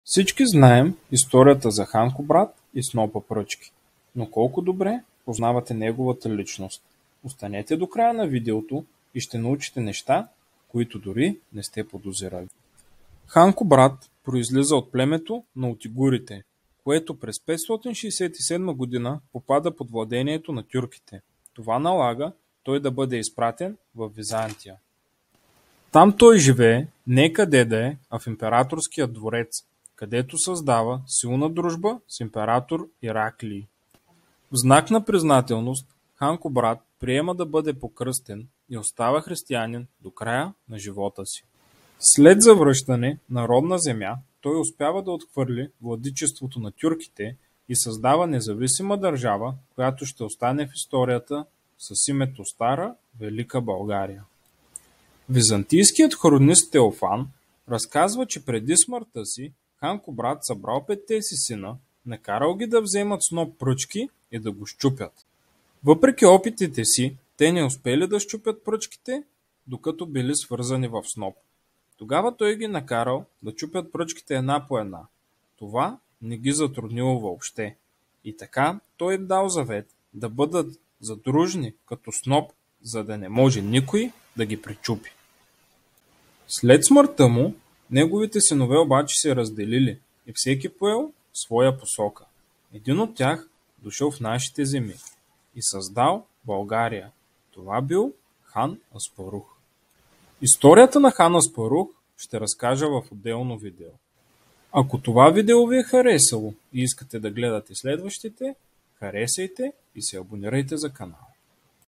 Невероятната история на Хан Кубрат
Историята на създателят на Стара Велика България- Хан Кубрат.